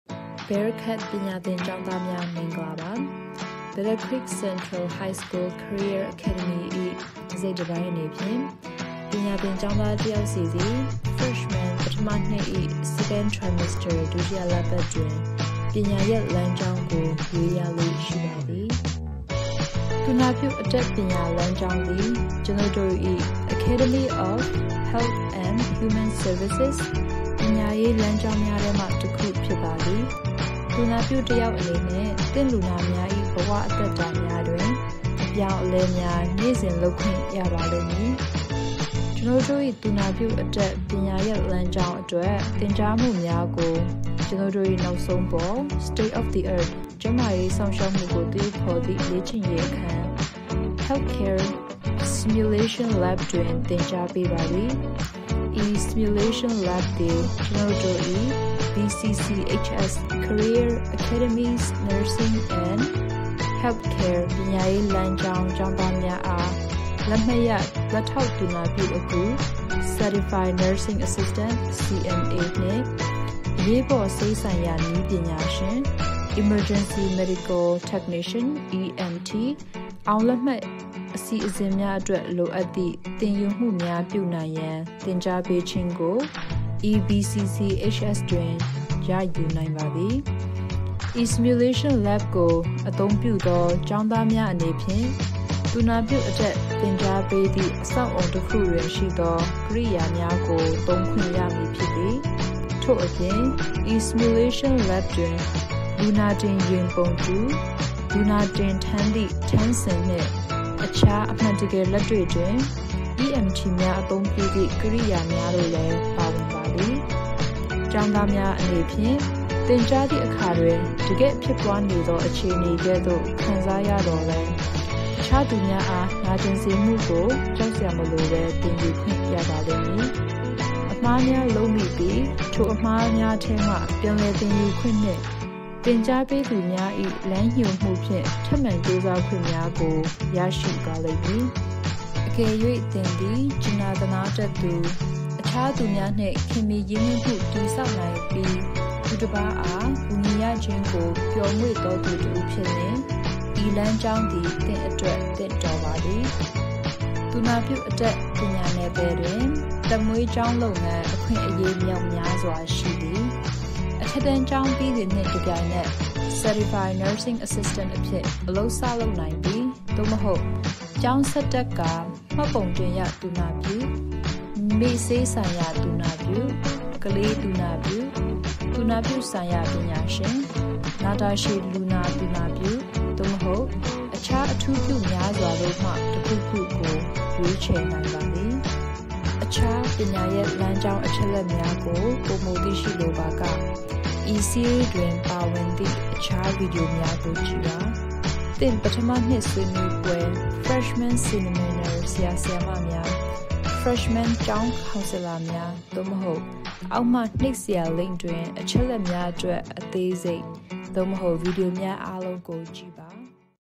Nursing Pathway (Burmese)
As part of the Battle Creek Central High School Career Academies, each scholar chooses a pathway during Semester 2 of their Freshman year. One of the pathways in our Academy of Health and Human Services is the Nursing pathway. As a nurse, you will be able to make a difference every day in the lives of your patients.